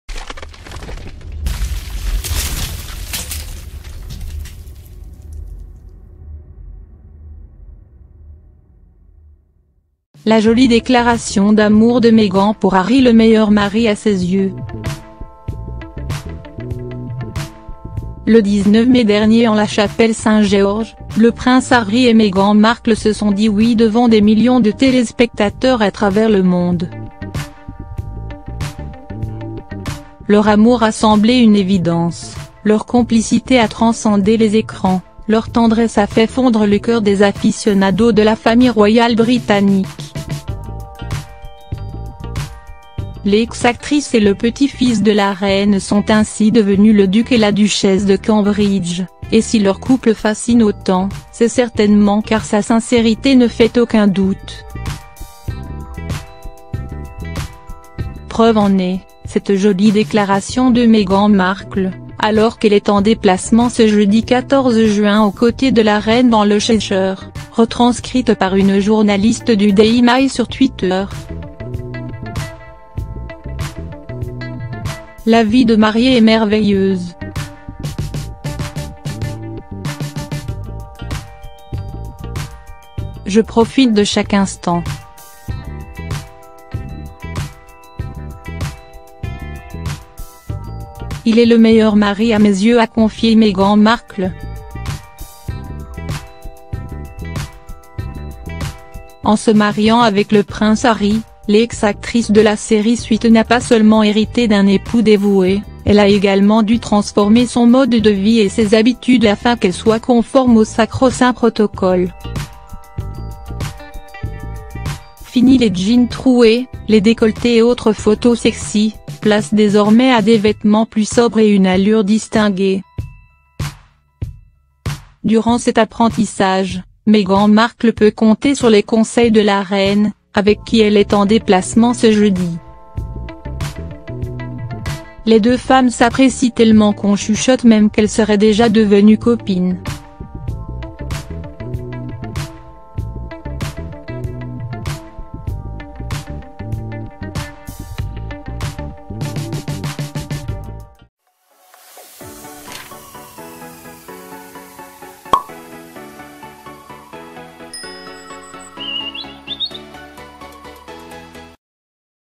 La jolie déclaration d’amour de Meghan pour Harry “le meilleur mari” à ses yeux
Meghan Markle est en déplacement avec la reine ce jeudi 14 juin. Un voyage que les deux femmes font seules, sans le prince Harry, ce qui n'empêche pas la jeune mariée de penser très fort à lui…